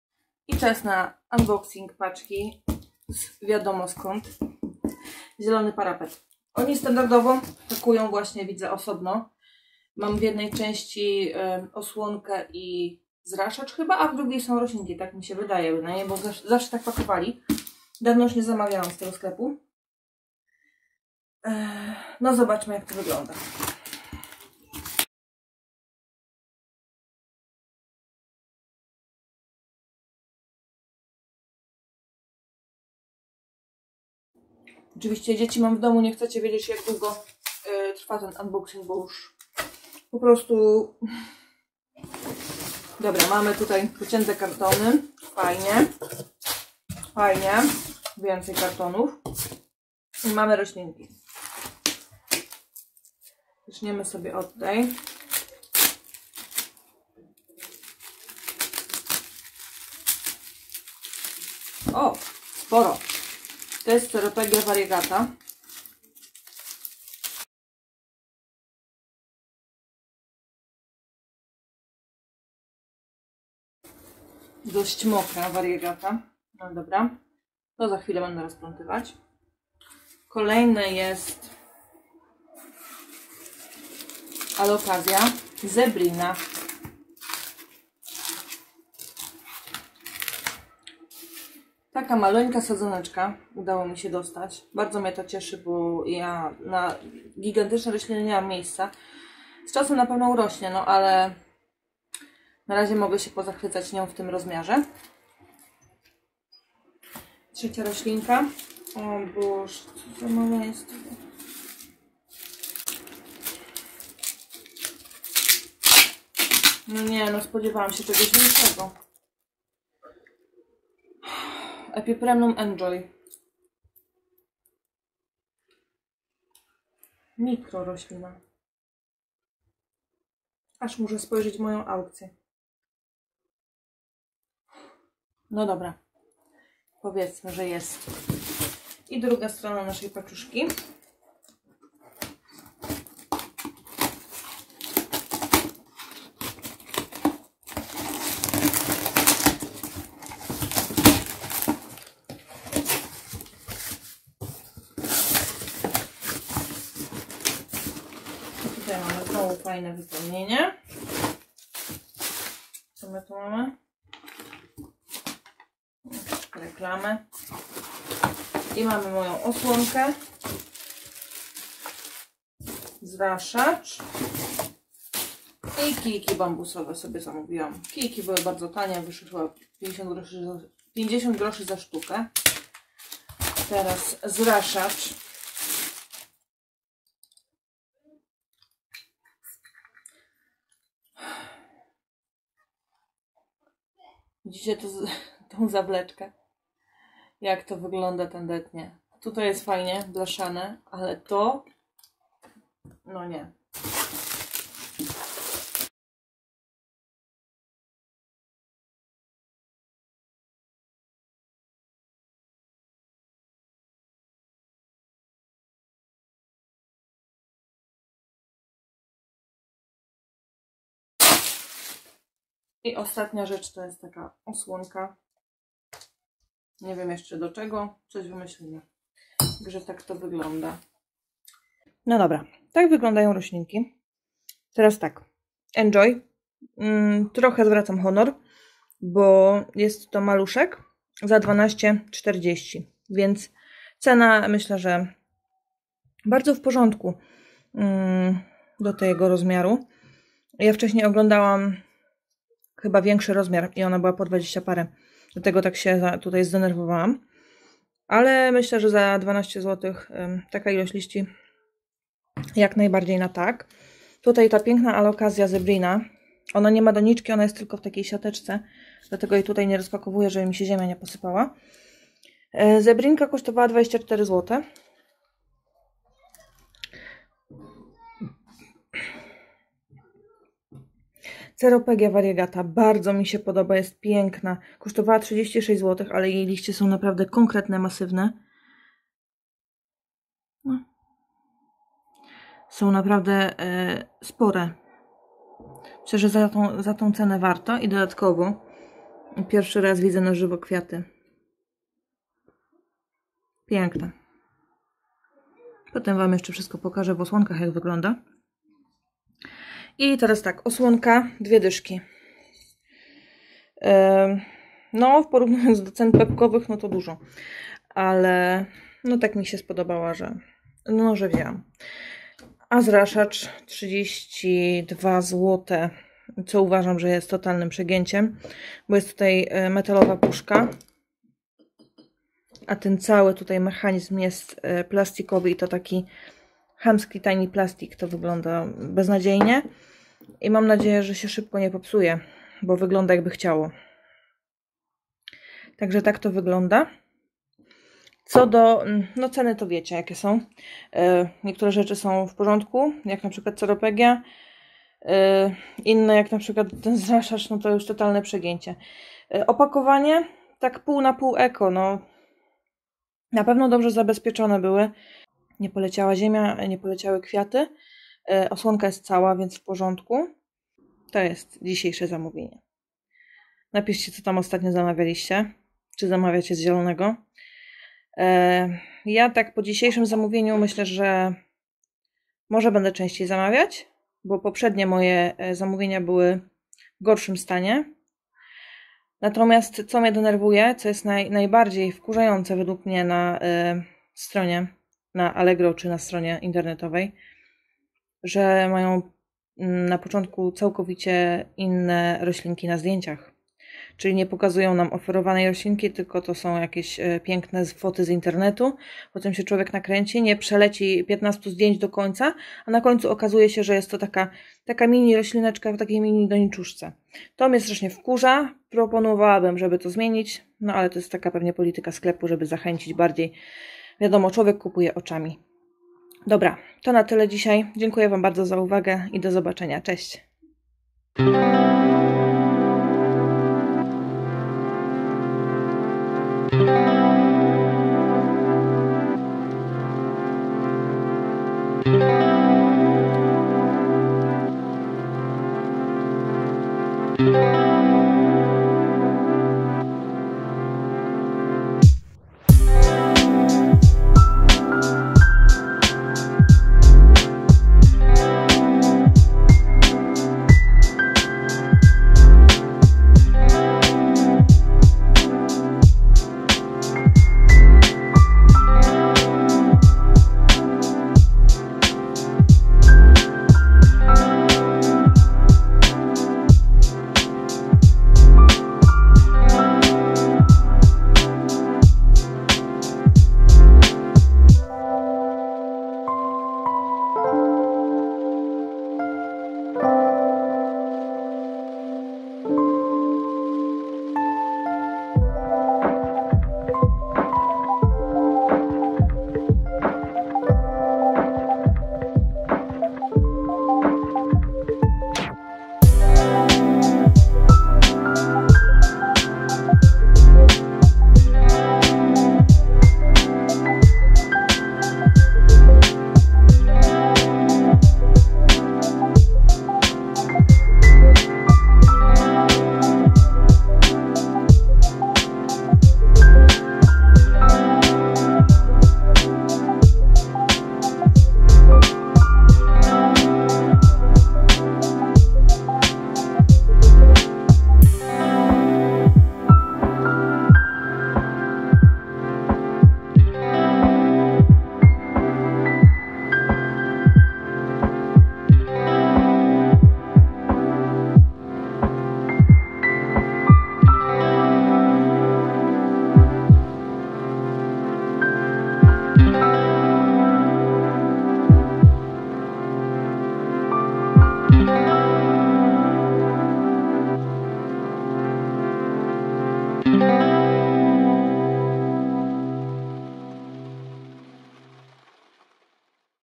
Uboxing * Rośliny z ZIELONY PARAPET * pakowanie, cena, jakość
Witajcie !
Celowo film pojawią się 2 tygodnie od otrzymania paczki,  niestety epipremnum njoy juz wylądowało na kompostowniku- zgniło ... Alokazja zebrina i ceropegia variegata jak narazie mają się dobrze .
Poraz kolejny się zawiodłam , juz kiedyś miałam z zielonego parapetu rosliny,  które bardzo szybko umarły jak np epipremnum golden photos , ceropegia silver glory czy syngonium Regina red ...
Zraszacz jeszcze działa 😉

Zapraszam na moje insta!
@bobiczkova_ - makijaż i kosmetyki
@bobiczkova_aparatka - profil fotograficzny
@bobiczkova_tworzy - rękodzieło, malarstwo, wnętrza, rośliny